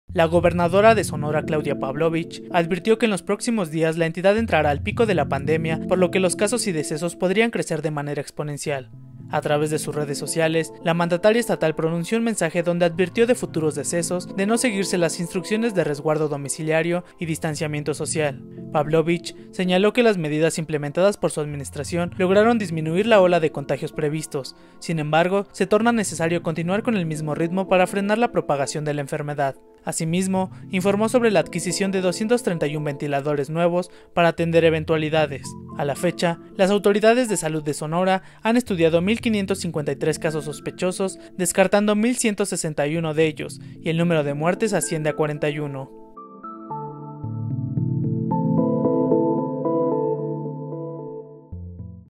Advierten en Sonora próximos días complicados por la pandemia
La gobernadora de Sonora, Claudia Pavlovich, advirtió que en los próximos días la entidad entrará al pico de la pandemia, por lo que los casos y decesos podrían crecer de manera exponencial.